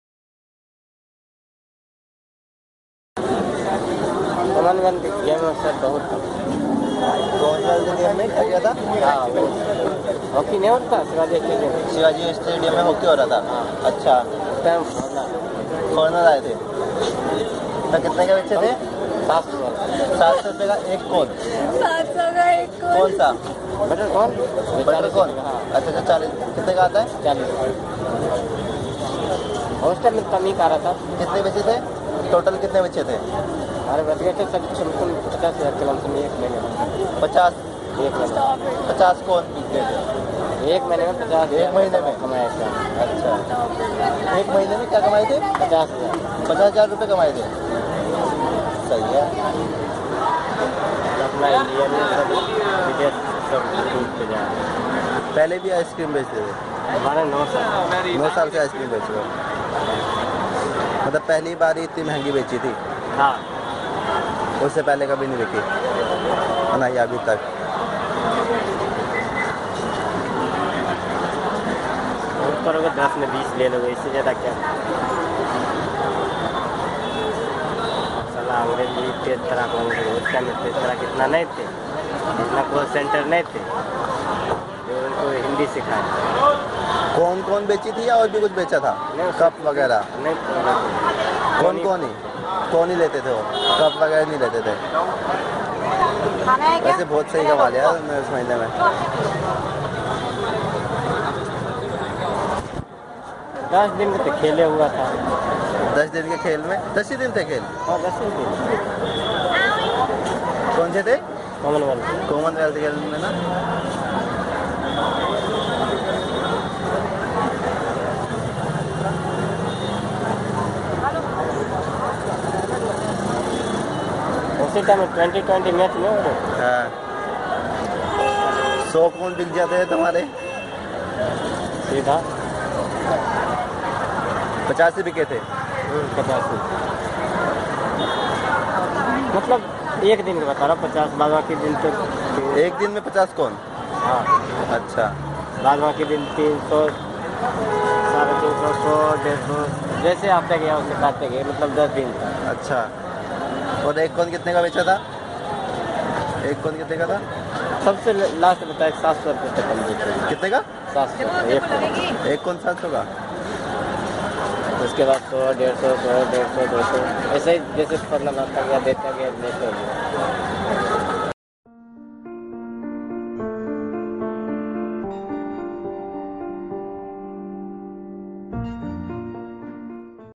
Foreigners cheated by Indian people during the Commonwealth Games(2010)
This is a clip of an ice-cream seller who confessed about how he cheated the foreigners during the Commonwealth Games which were held in India in 2010. Even today the foreigners are being cheated in many ways. Whether it be any small vendor or a branded showroom, they are fooled. They are even misguided and cheated by their own guides. Please do not cheat anyone if he/she is not well awared. Let the beautiful side of our beautiful country be potrayed to all. India is known for its vivid culture and beauty and lets maintain that. Thank you!